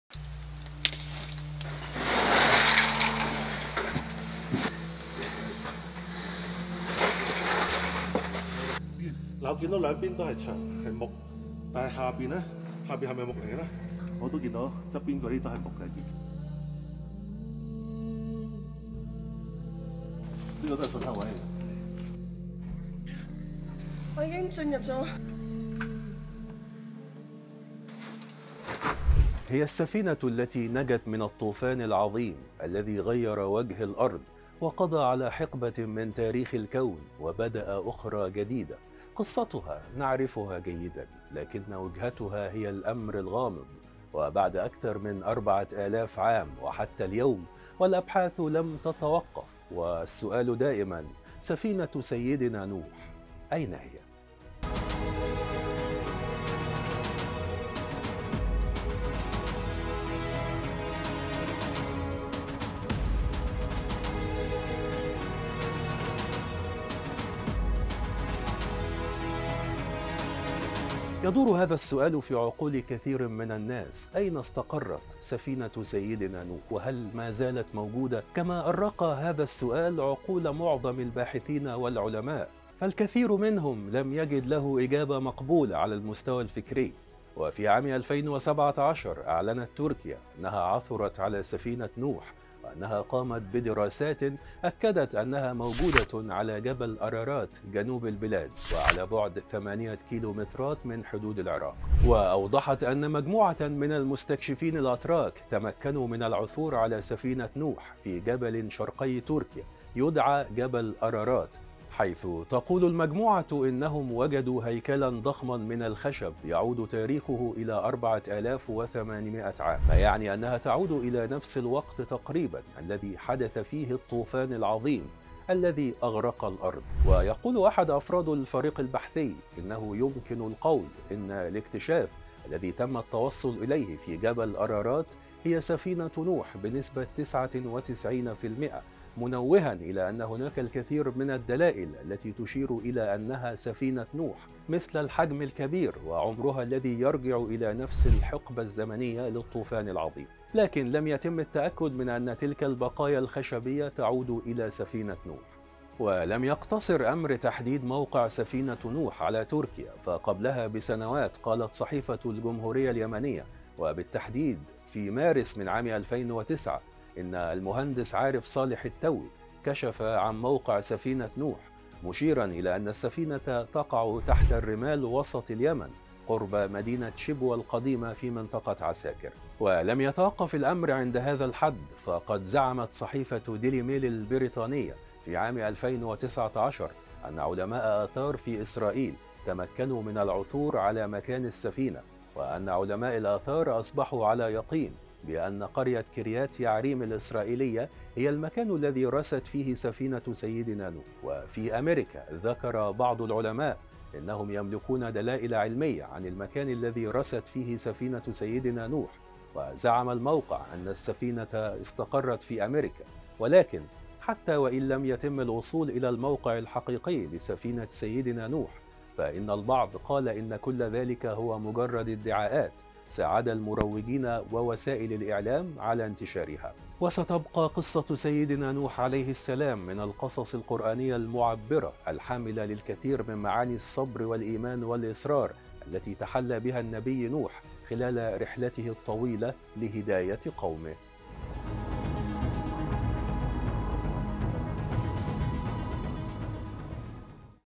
تخفي أسرار الكون والحياة!مفاجأة تركية للعالم تكتب تاريخا جديدا.. هل عثر على سفينة نوح؟ السر العظيم !
في عام 2017 أعلنت تركيا أنها عثرت على سفينة نوح، وأنها قامت بدراسات أكدت أنها موجودة على جبل أرارات جنوب البلاد على بعد 8 كم من حدود العراق. وأوضحت أن مجموعة من المستكشفين الأتراك تمكنوا من العثور على سفينة نوح، في جبل شرقي تركيا يدعى جبل أرارات  حيث تقول المجموعة إنهم وجود هيكل ضخم من الخشب يعود تاريخه إلى 4,800 عام